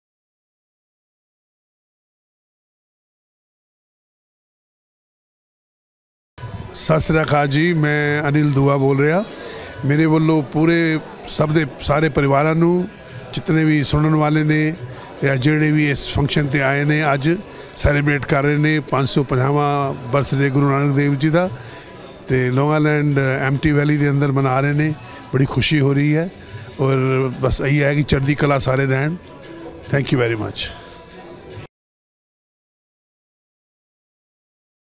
SCIENCE OF SPIRITUALITY 550th BIRTH ANNIVERSARY OF SHRI GURU NANAK DEV JI 2019.||4K OFFICIAL VIDEO
550th BIRTH ANNIVERSARY OF SHRI GURU NANAK DEV JI2019 ( SAWAN KIRPAL RUHANI MISSION ) || 4K OFFICIAL VIDEO ||
PLACE:- SCIENCE OF SPIRITUALITY MEDITATION CENTER. 79 COUNTY LINE ROAD, AMITYVILLE, NY-11701.
PHONE NO:- ENGLISH:- (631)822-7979, HINDI:- (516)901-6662.